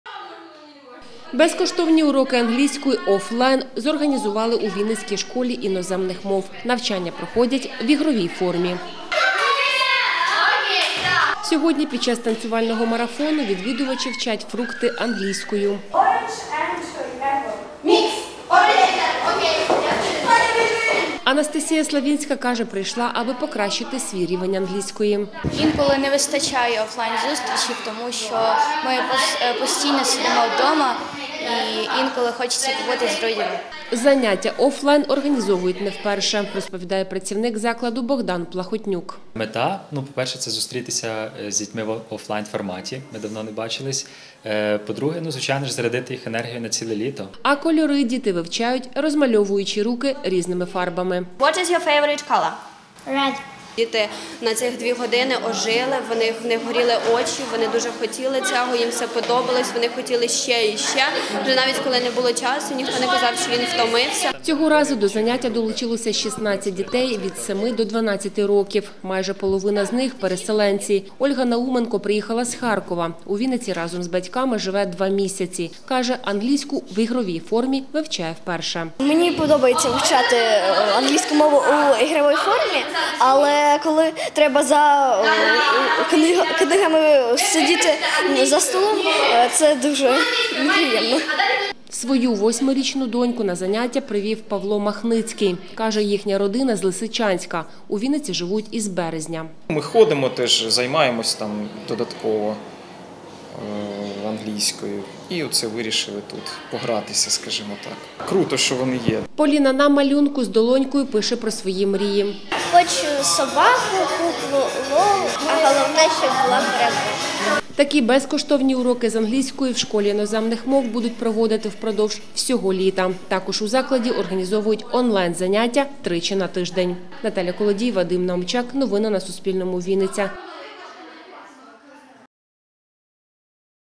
З танцями та іграми: у Вінниці дітей безкоштовно навчають англійської
Безкоштовні уроки англійської офлайн зорганізували у вінницькій школі іноземних мов. Навчання проходять в ігровій формі. За словами працівника закладу Богдана Плахотнюка, заняття офлайн організовують не вперше.